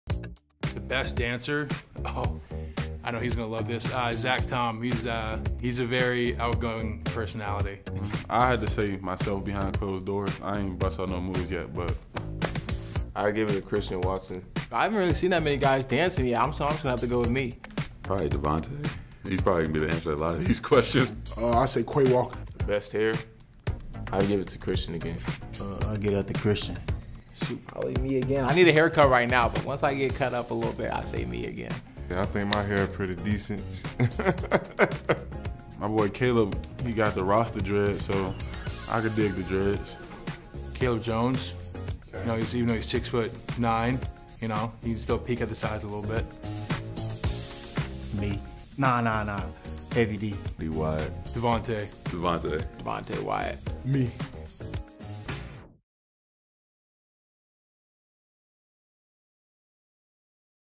Rookie Ramblings: Best in class
The Green Bay Packers rookies crown the best of the best in this year's class.